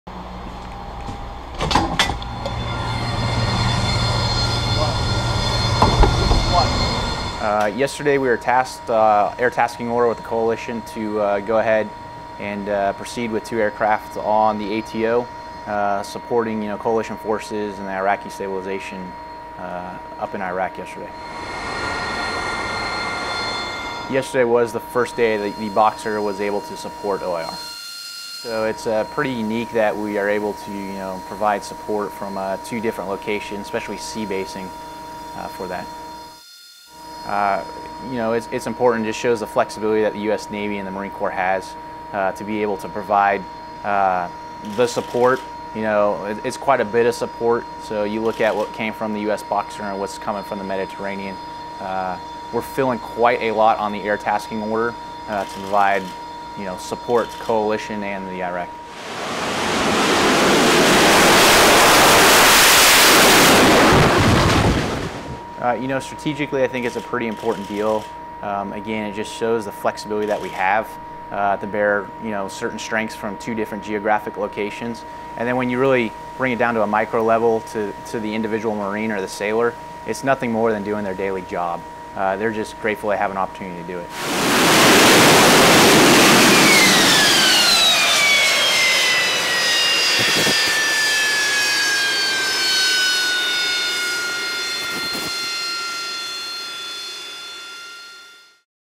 Marine Harriers on the USS Boxer
Check out the flight deck of USS Boxer the moments before Boxer and the 13th Marine Expeditionary Unit made history by launching its first airstrike sorties against the Islamic State in Iraq and Syria, marking the first time during Operation Inherent Resolve the U.S. Navy has launched ship based airstrike sorties from both the Mediterranean and the Persian Gulf simultaneously.